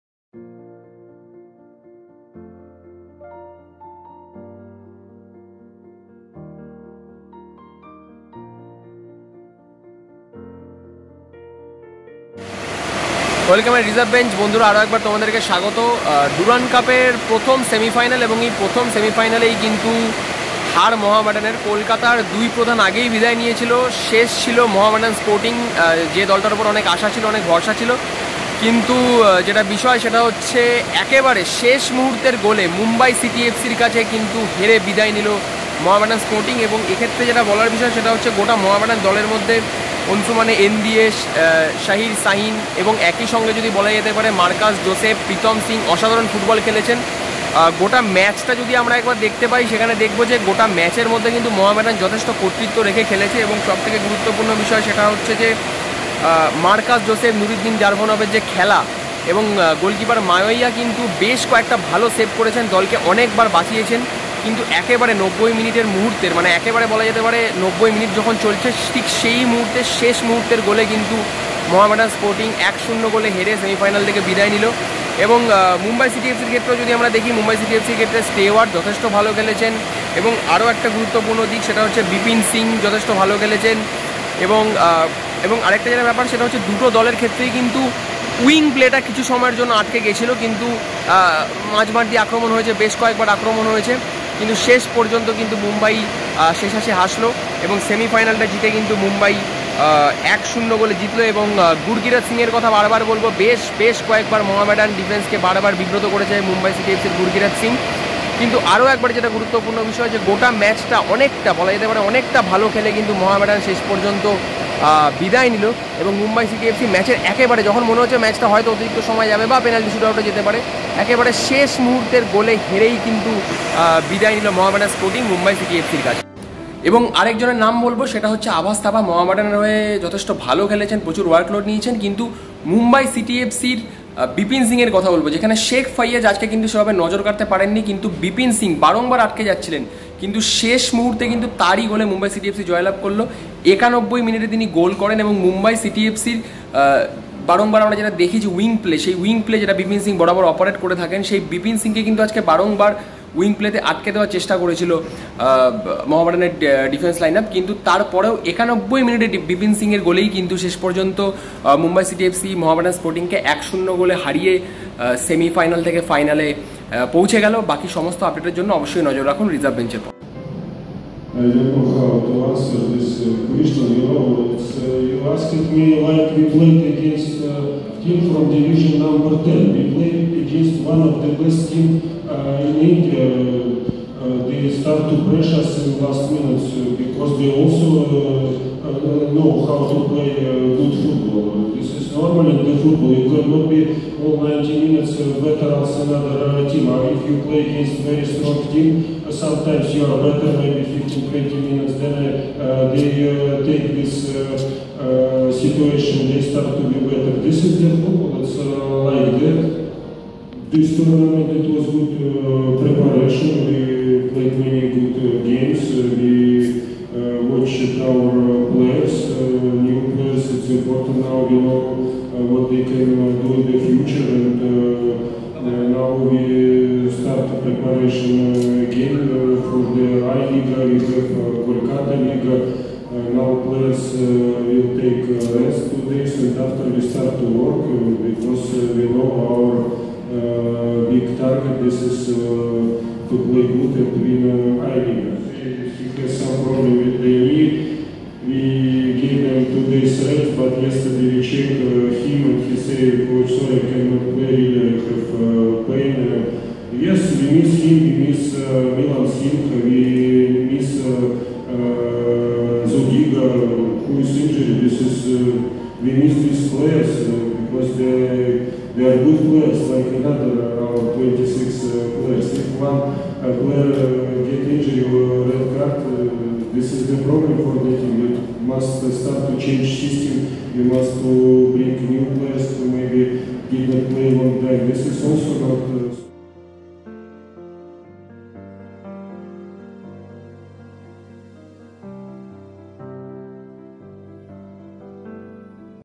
Mohammedan Sporting vs Mumbai City FC, Post-Match Review & Press Conference | Durand Semifinal
Mohammedan Sporting vs Mumbai City FC, Post-Match Review & Press Conference..